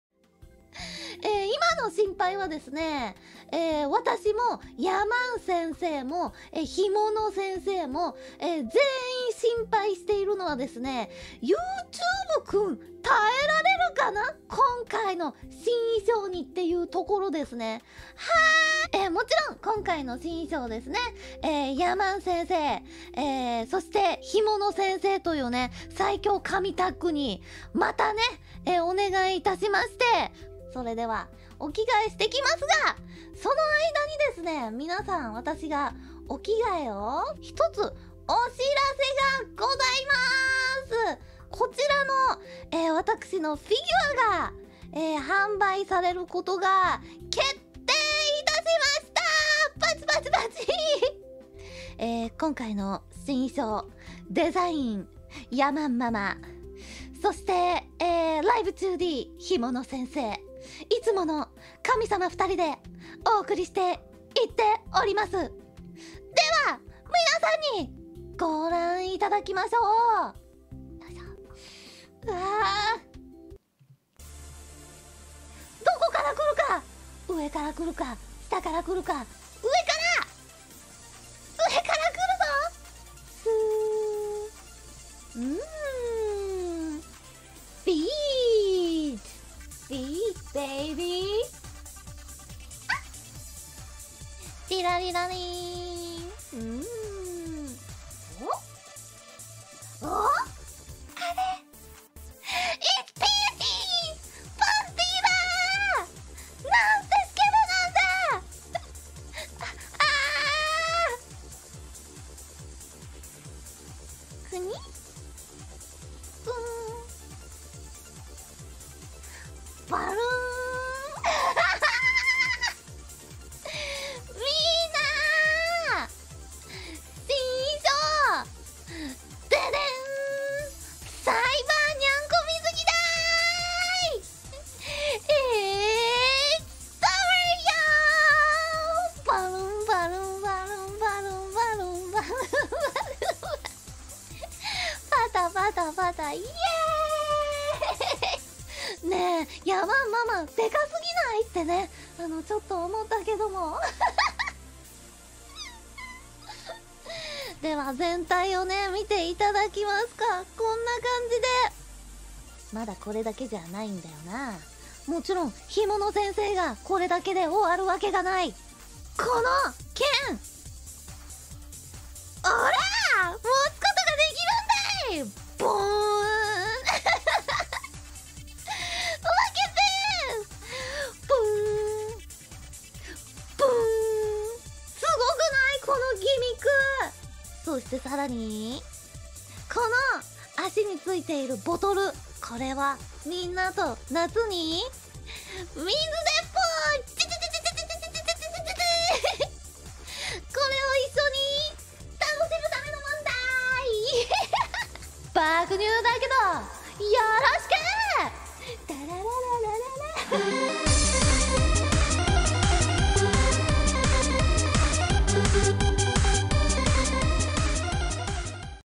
Kson se preocupa antes de estrenar su nuevo traje y da pequeño anuncio mientras se cambia [Sub Esp]
Figura de Kson